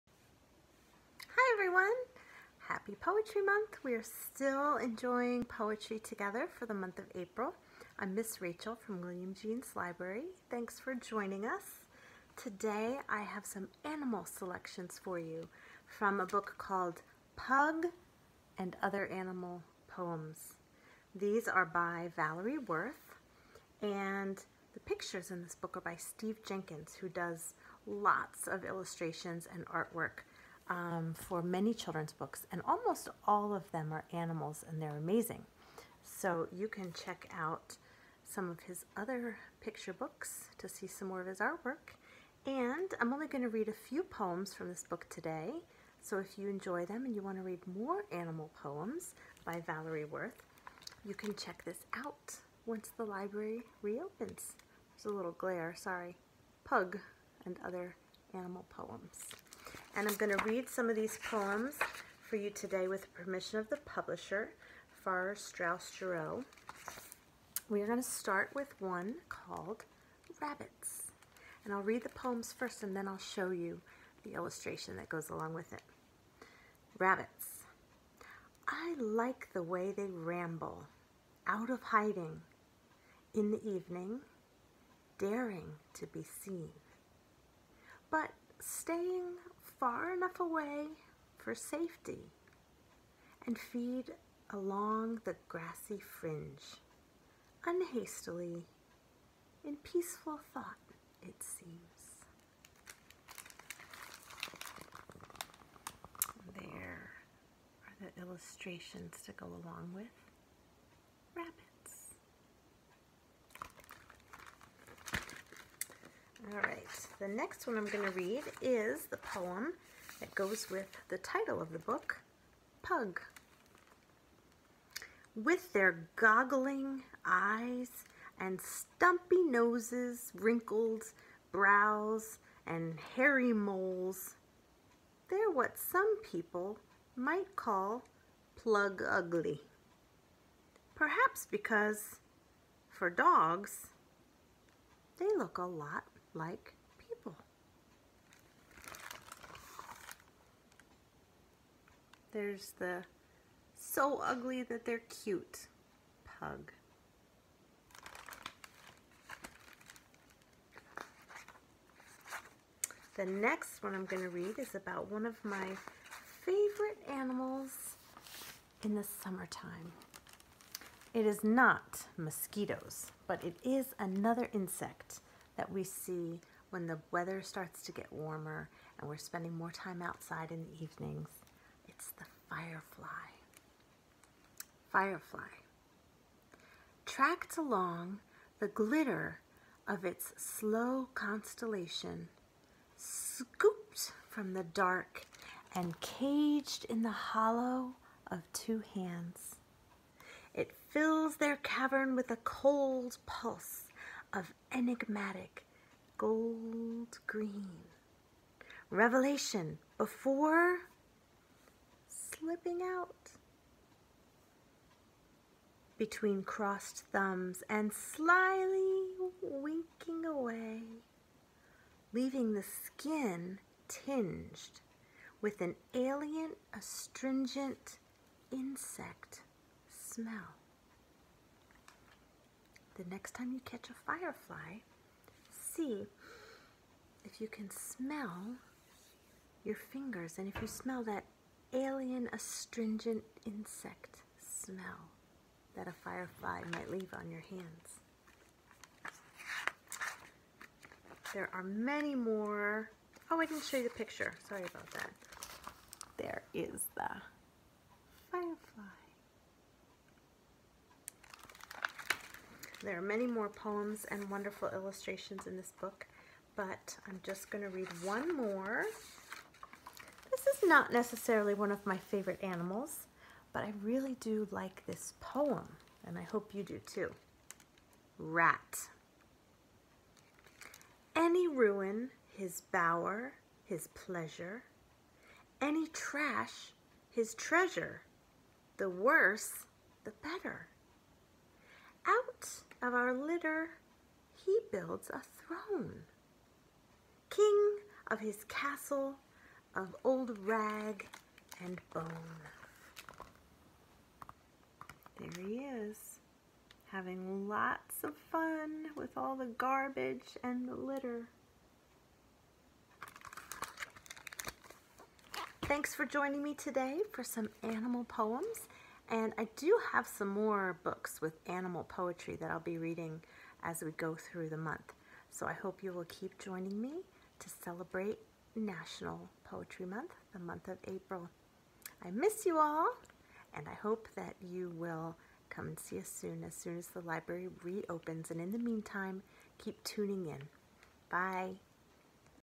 4 selections from "Pug and Other Animal Poems" by Valerie Worth
Read by Ms. Rachel with permission from Farrar Straus Giroux.
 "Rabbits"
"Pug"
"Firefly"
"Rat"